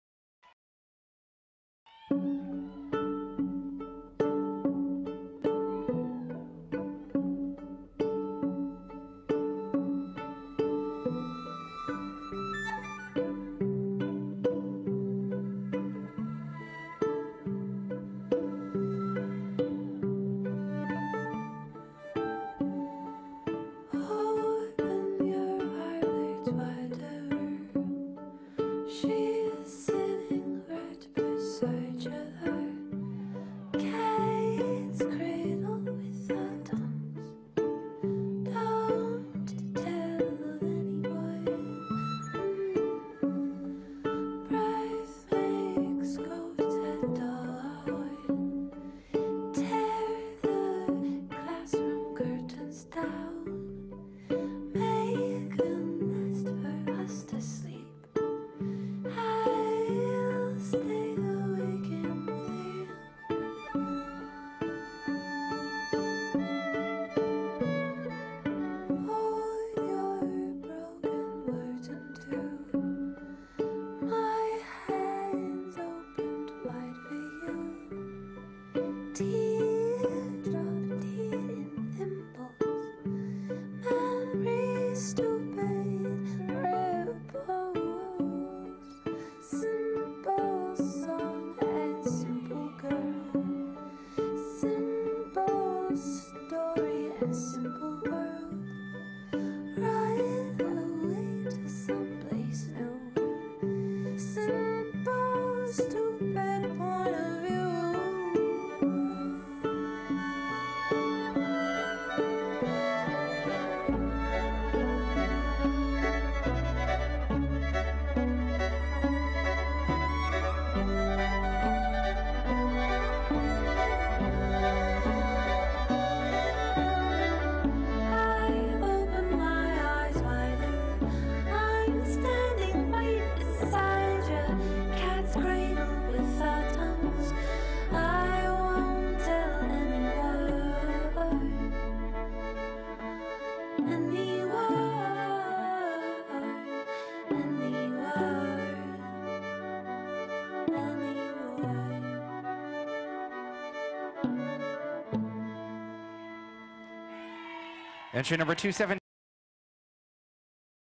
Izzy Pereira Senior Contemporary Solo - A Simple Point of View
2nd Overall Senior Solo, 24seven Pittsburgh, Stop the Clock, Senior Non-Stop Dancer Winner
Choreographed by Teddy Forance
Studi Performing Dance Arts